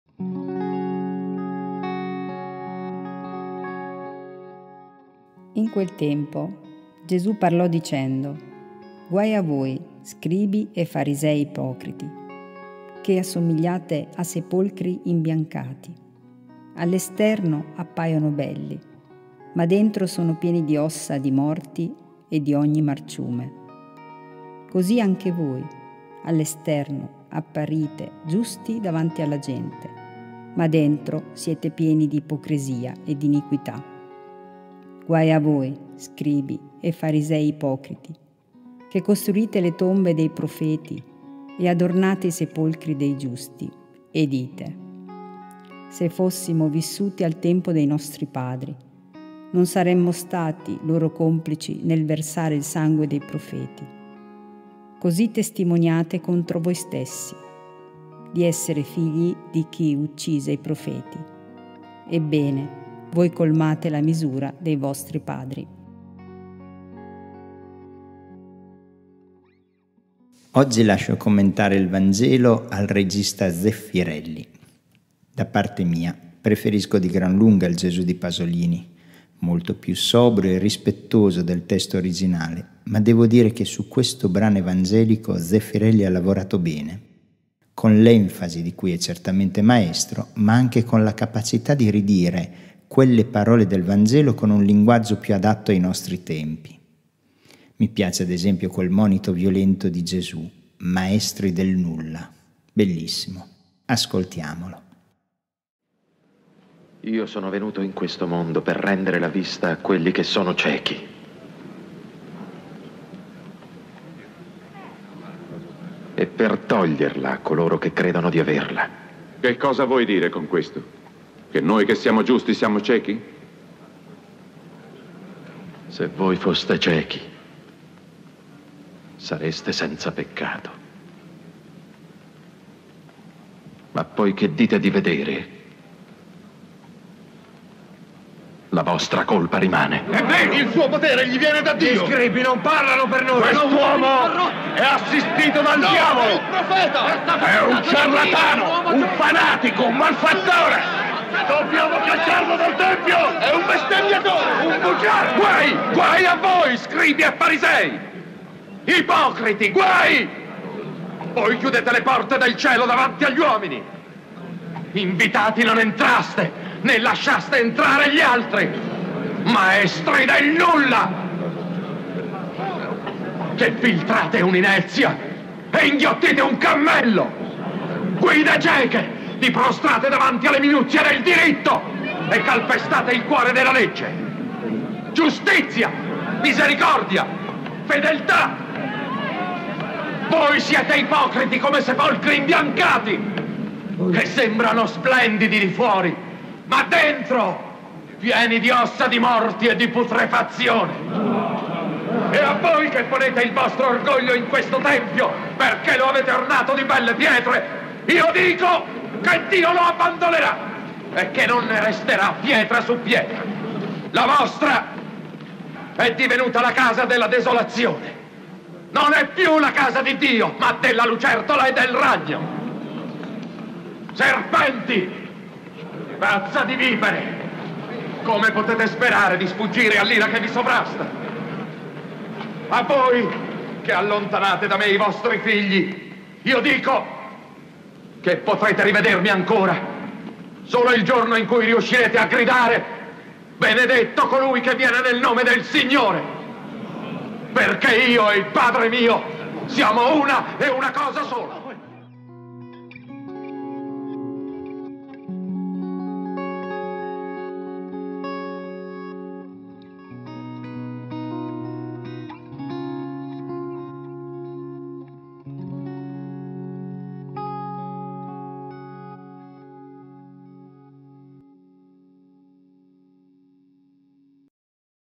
Vangelo del 30 Agosto 2023: Matteo 23, 27-32 con il commento di don Franco Mastrolonardo
In quel tempo, Gesù parlò dicendo: «Guai a voi, scribi e farisei ipocriti, che assomigliate a sepolcri imbiancati: all’esterno appaiono belli, ma dentro sono pieni di ossa di morti e di ogni marciume. Così anche voi: all’esterno apparite giusti davanti alla gente, ma dentro siete pieni di ipocrisia e di iniquità.
Guai a voi, scribi e farisei ipocriti, che costruite le tombe dei profeti e adornate i sepolcri dei giusti, e dite: “Se fossimo vissuti al tempo dei nostri padri, non saremmo stati loro complici nel versare il sangue dei profeti”. Così testimoniate, contro voi stessi, di essere figli di chi uccise i profeti. Ebbene, voi colmate la misura dei vostri padri».